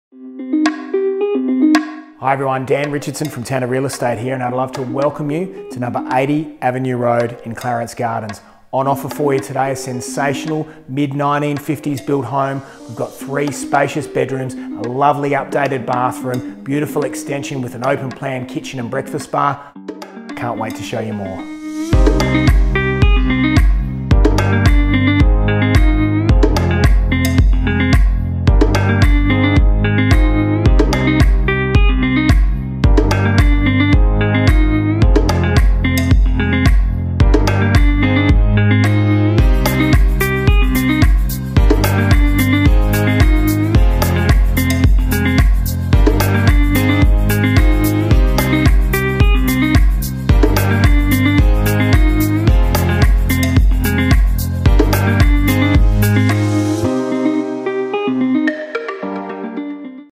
80 Avenue Road, Clarence Gardens
Dan Richardson from Tanner Real Estate presents this lovely home in Clarence Gardens, South Australia.